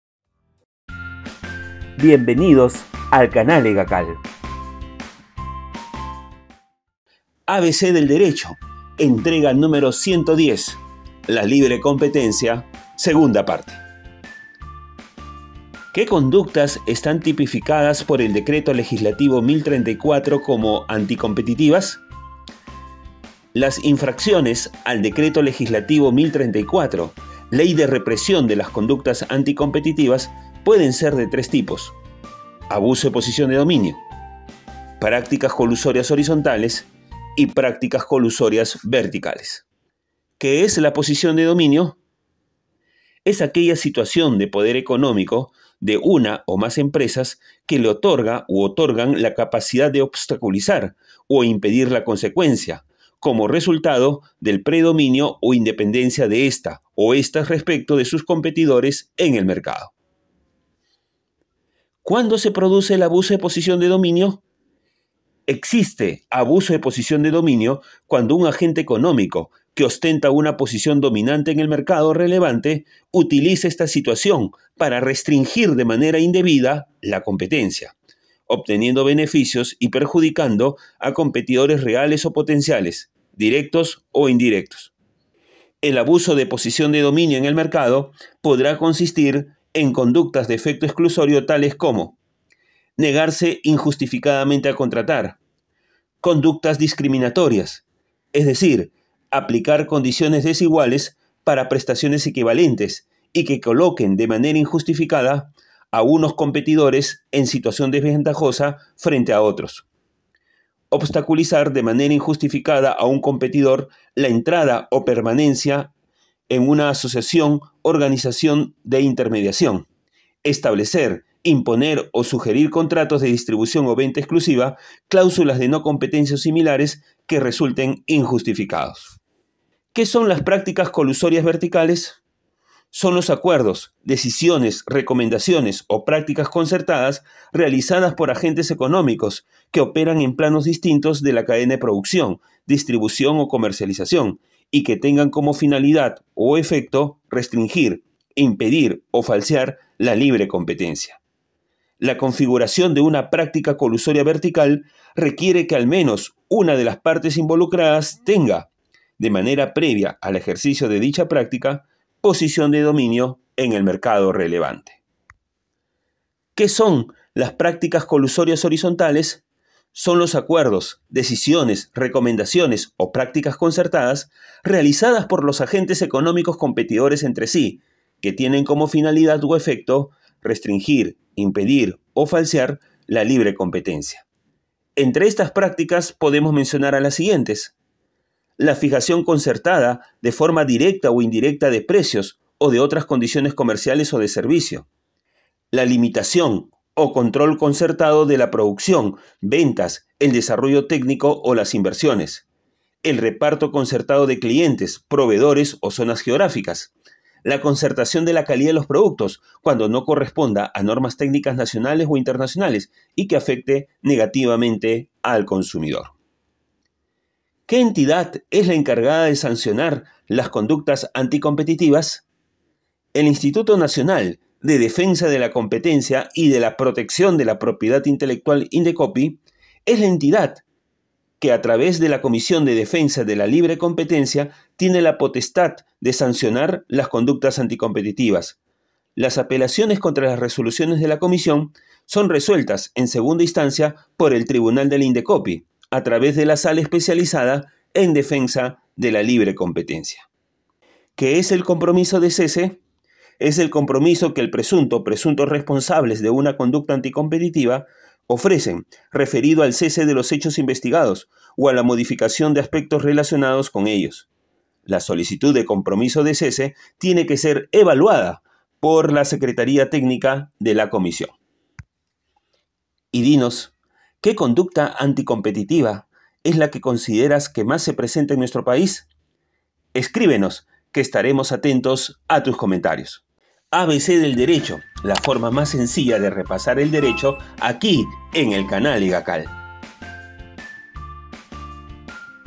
LA LIBRE COMPETENCIA (Parte 2): Posición de dominio. Conductas anticompetitivas| ABC del Derecho 110
En este entrega "LA LIBRE COMPETENCIA (Parte 2)" responderemos las siguientes interrogantes: 6. ¿Qué conductas están tipificadas por el Decreto Legislativo N° 1034, como anticompetitivas? | 7. ¿Qué es la posición de dominio? | 8. ¿Cuándo se produce el abuso de posición de dominio? | 9. ¿Qué son las prácticas colusorias verticales? | 10. ¿Qué son prácticas colusorias horizontales? | 11. ¿Qué entidad es la encargada de sancionar las conductas anticompetitivas?